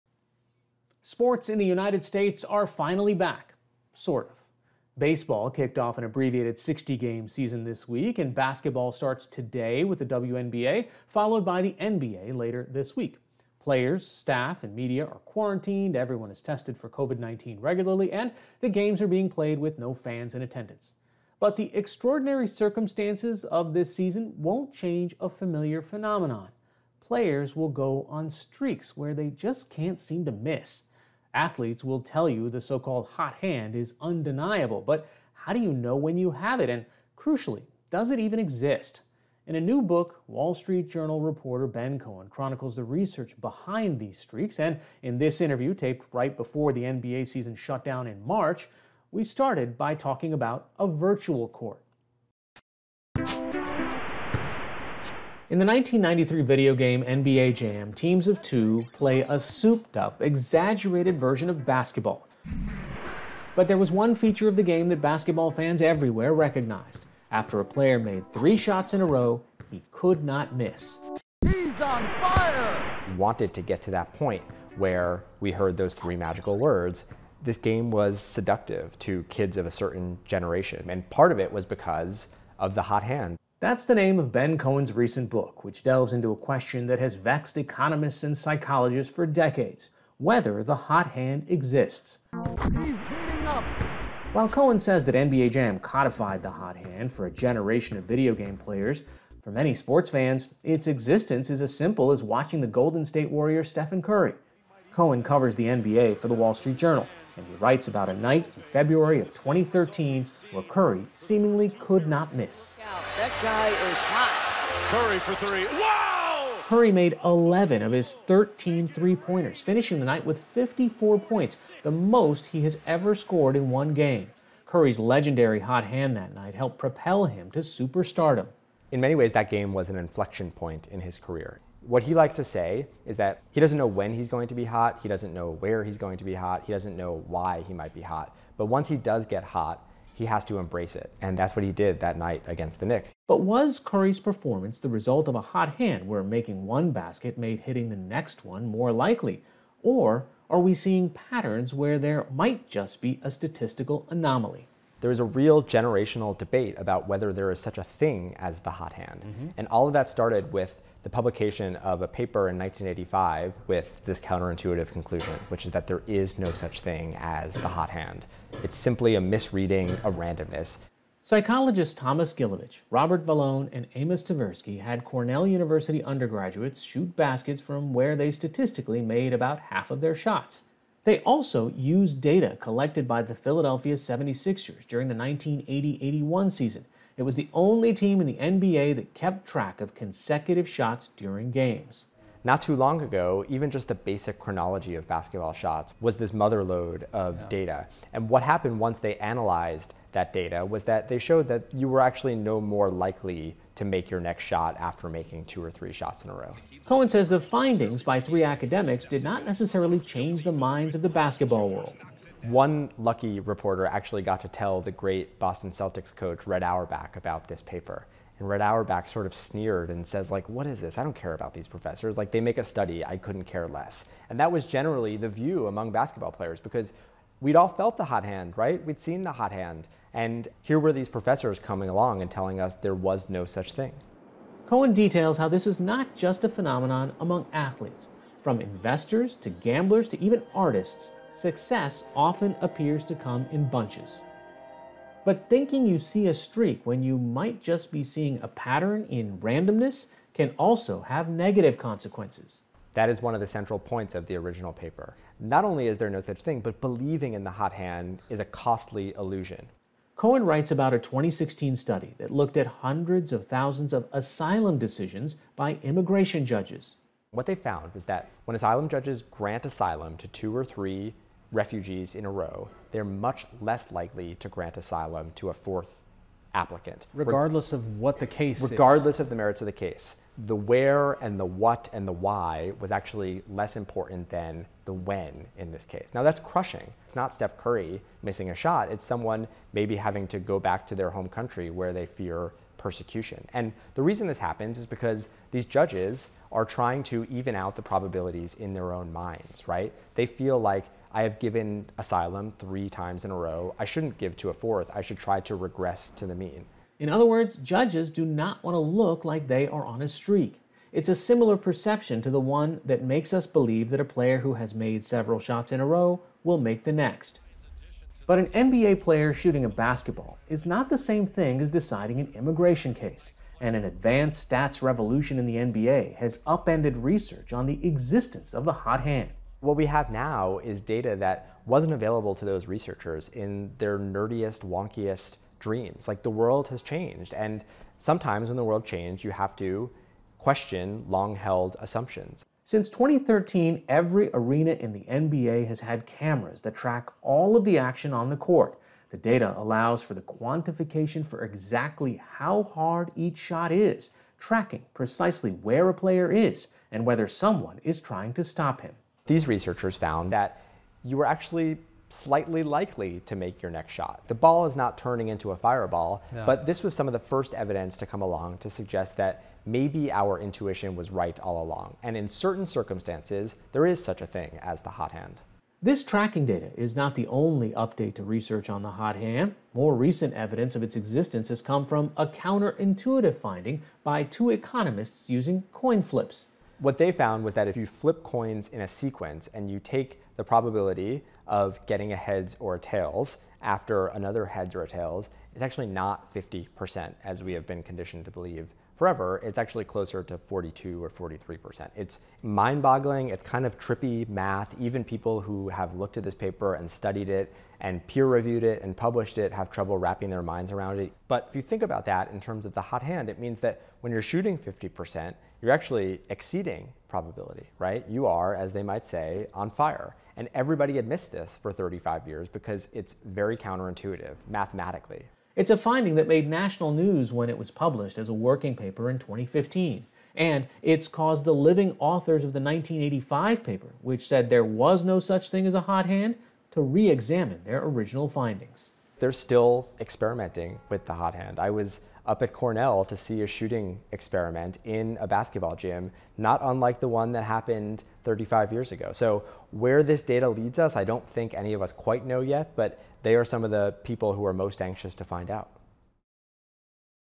The science behind streaks on and off the basketball court
In the seminal arcade video game, NBA Jam, a player making two shots in a row would begin "heating up," and after three shots they would go on an unstoppable shooting streak. But do these kinds of streaks exist in real life? Hari Sreenivasan spoke with Wall Street Journal reporter Ben Cohen about his new book, "The Hot Hand," and the implications of streaks both on and off the basketball court.